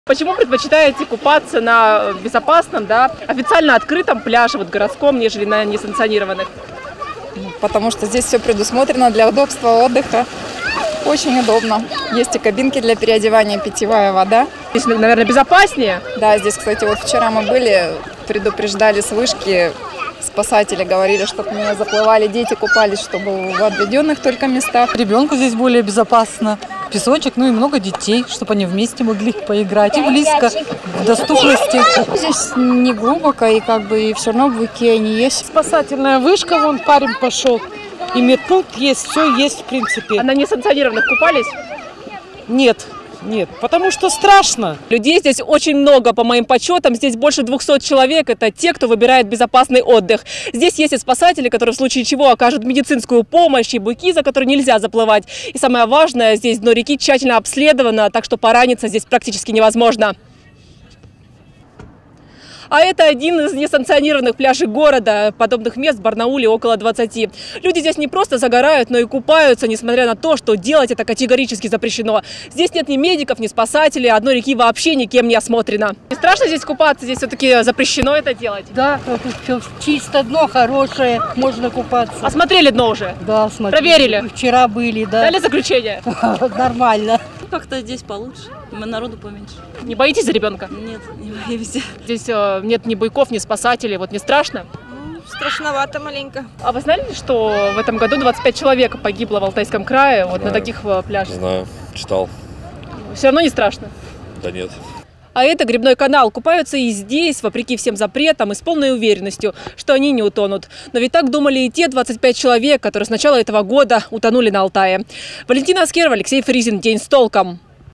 Чем опасны стихийные пляжи и почему барнаульцы выбирают рискованный отдых?
Погода наконец-то наладилась. По заверению  спасателей на территории края официально открыто 55 мест для купания.
Наши корреспонденты сегодня решили проверить,  какие места для купания выбирают барнаульцы - безопасные или стихийные водоемы.